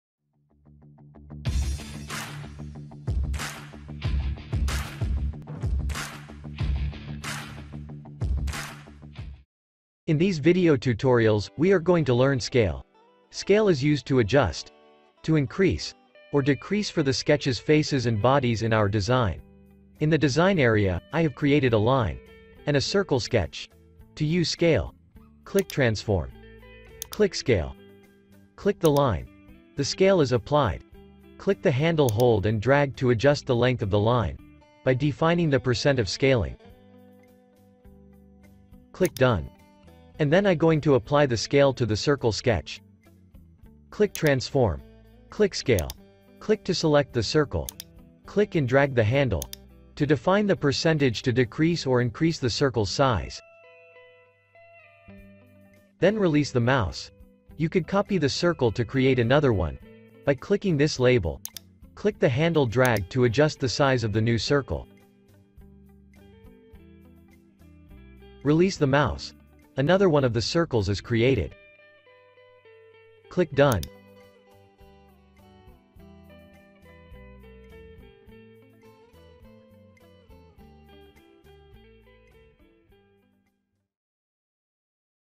Scale The Sketch
Hello everyone in this video tutorial, I am going to show you how to Scale The Sketch  in Shapr3d.

 solidworks